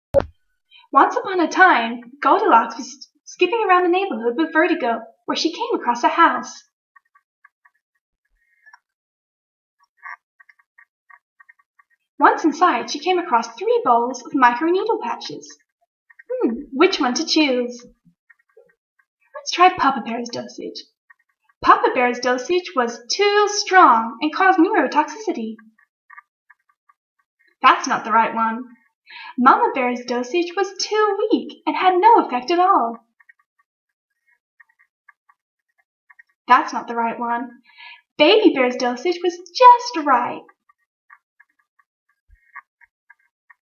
Goldilocks with Vertigo
Treating Goldilocks for Vertigo with microneedle patches